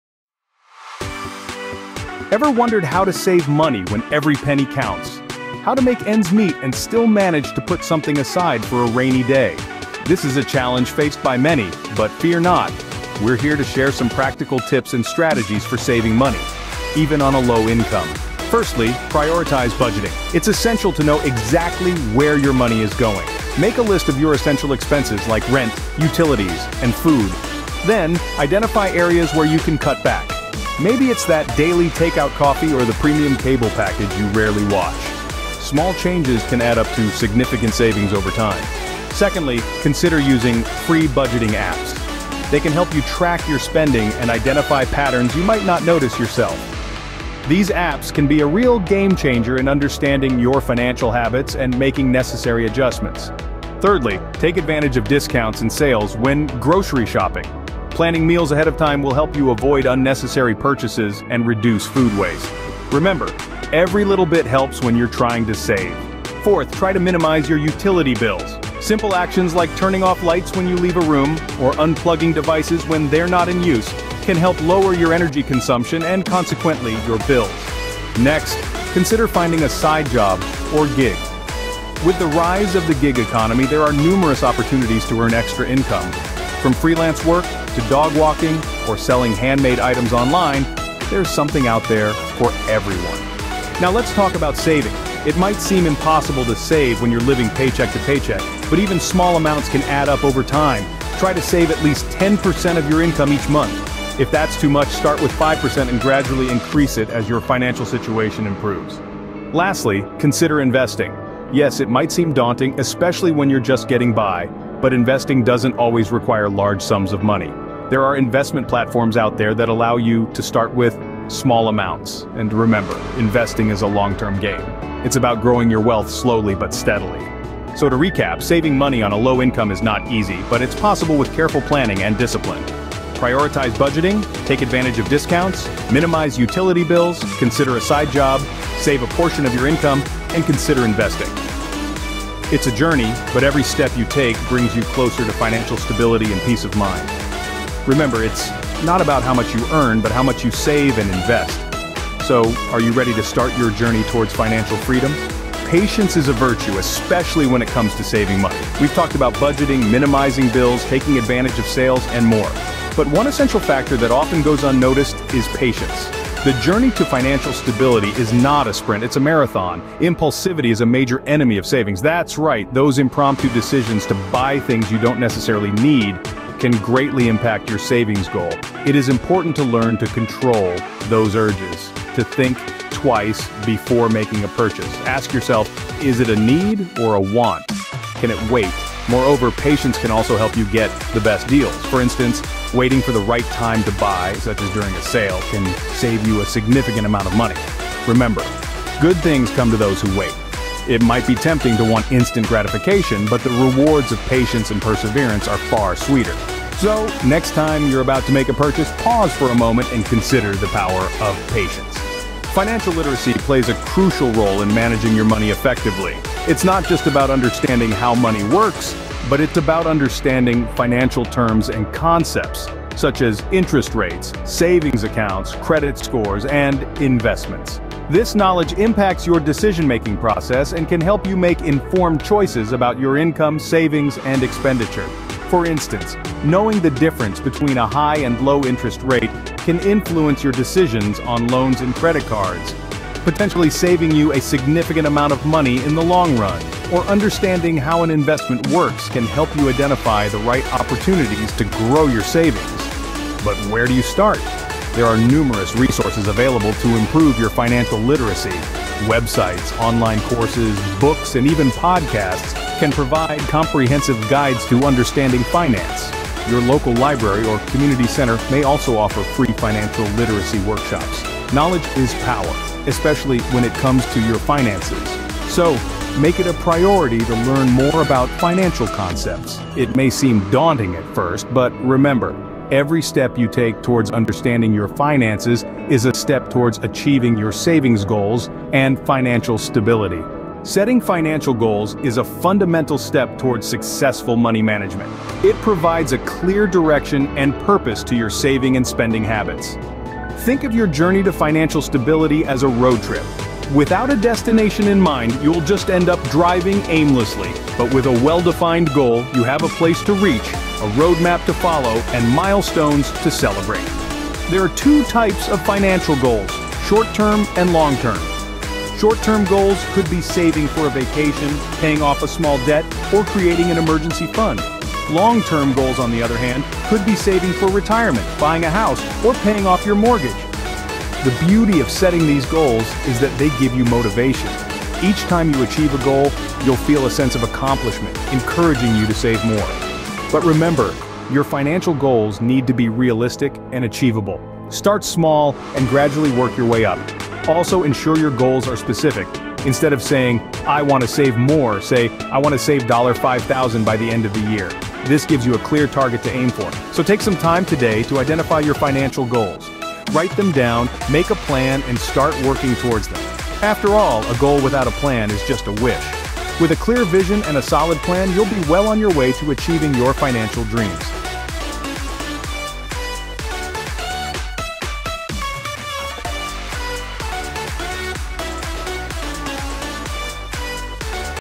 Maximize Savings on a Low Income
Everyone needs to save money, but how is it possible these days if you have low income?